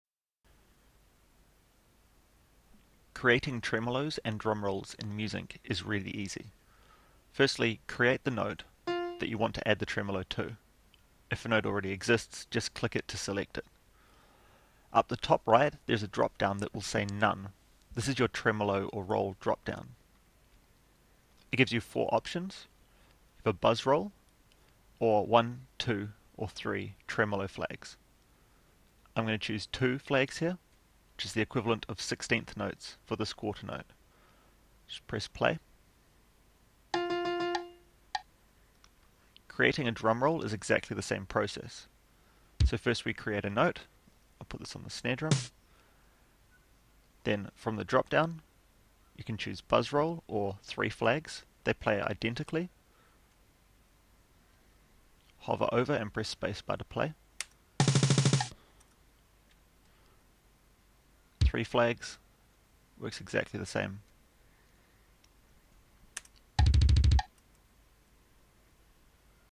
Micro-tutorial: Tremelo Flags and Drum Rolls in Musink (Free Notation Software)
How to create tremelos and drum rolls in Musink.

Musink is free music composition software that is very easy to use, and very fast to write with. It lets your create MIDI files, and beautiful sheet music for any instrument.